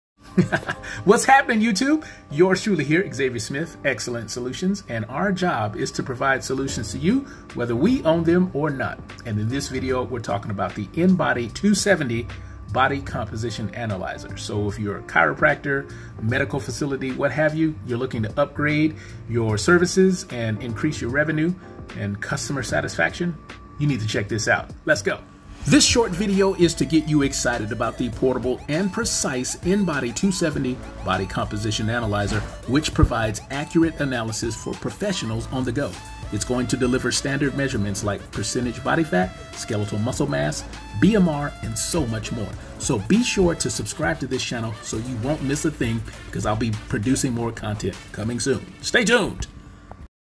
Inbody 270 Body Composition Analyzer - Great Solution for Professionals on the Go
Get excited about the Inbody 270 Composition Analyzer available through my online Veteran-Owned store!

It's hard to stay motivated when it comes to fitness, and even harder to track your progress.

You've probably been told that you need to track your body composition in order to see real results, but who has the time or money for that?

The InBody 270 is a portable and precise body composition analyzer that quickly and easily measures your fat mass, muscle mass, and body water.

With no empirical estimations based on age, sex, ethnicity, or body type, this device delivers accurate analysis for professionals on the go.